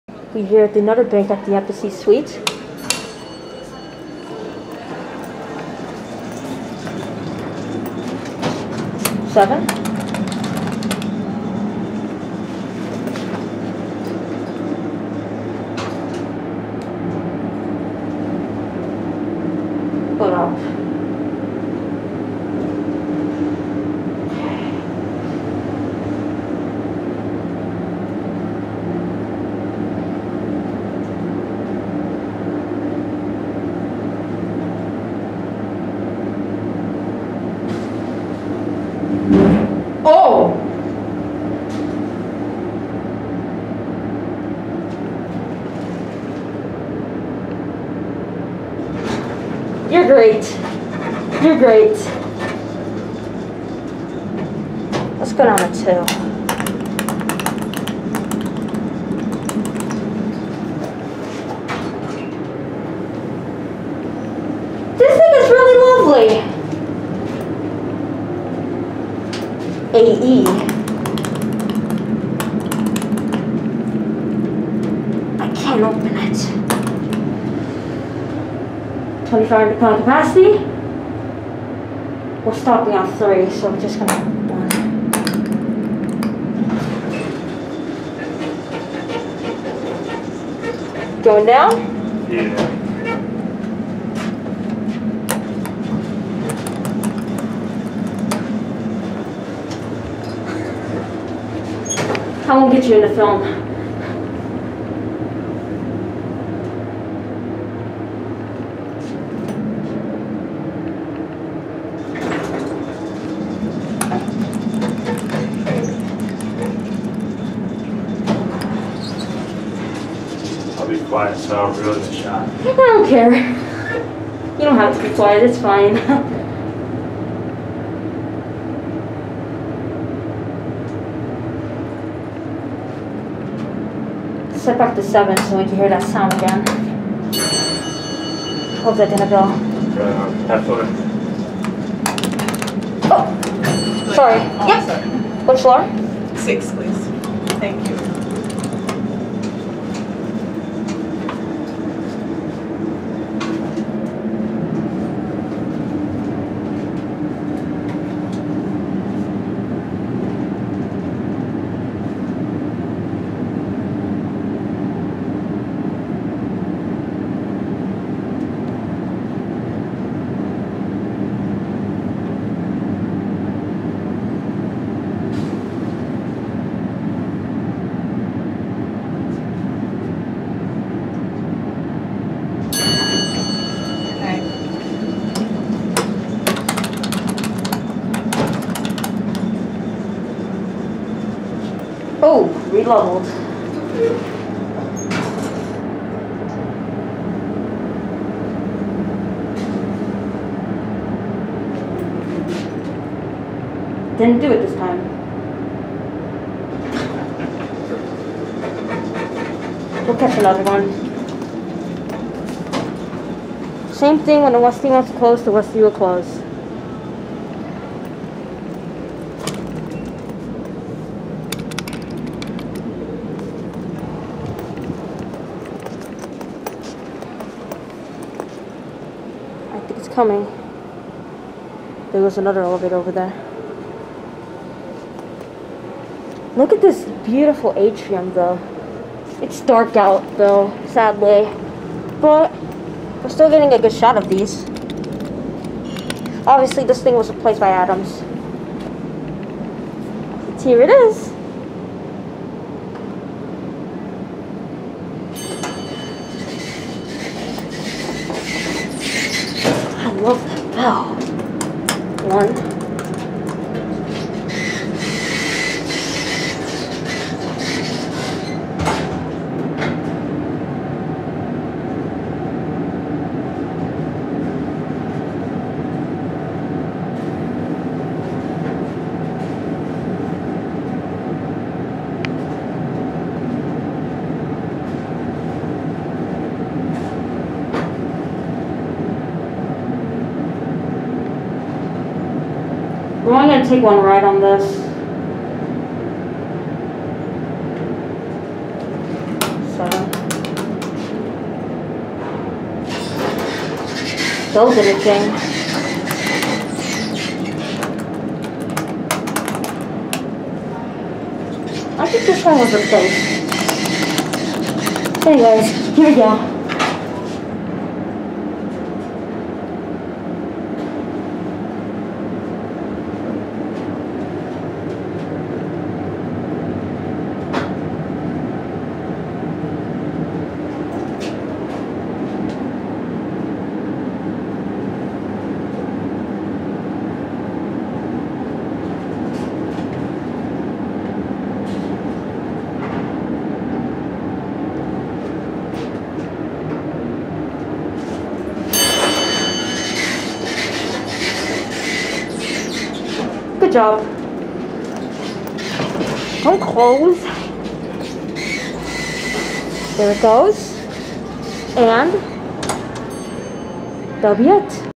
Bank 2 - Westinghouse AE HIGH Hydraulic elevator at Embassy Suites in Schaumburg IL
Filmed on June 25th

These elevators are amazing. These banks have different motors.